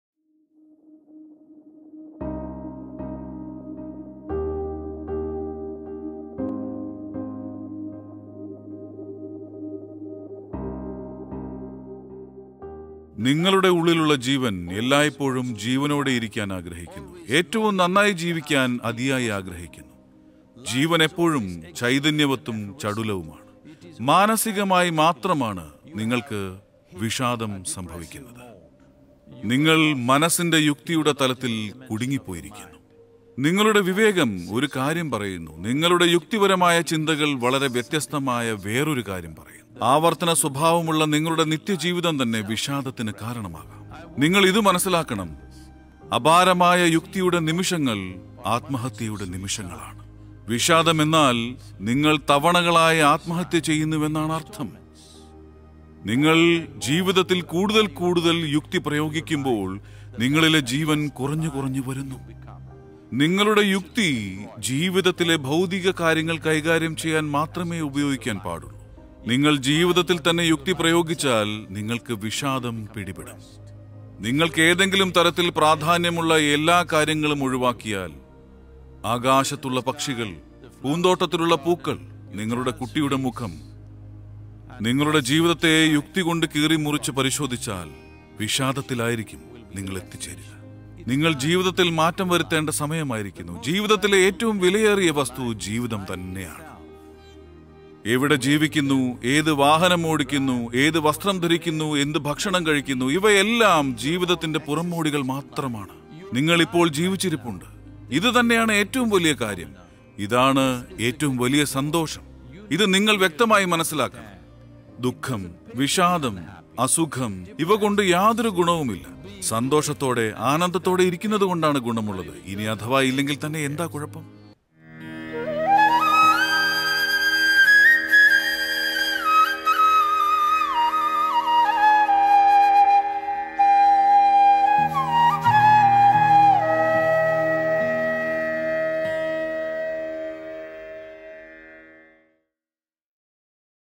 വിഷാദത്തില്‍ നിന്നും മുക്തി നേടാം | Sadhguru Malayalam
ഒരാള്‍ക്ക് നേരിടേണ്ടി വരുന്ന ഏറ്റവും വലിയ ശത്രുവാണ് വിഷാദം. ഈ പരുക്കന്‍ സത്യത്തെ കുറിച്ച് സദ്ഗുരു സംസാരിക്കുന്നു. എങ്ങനെ അതില്‍ നിന്നും രക്ഷ നേടാമെന്നും പറയുന്നു.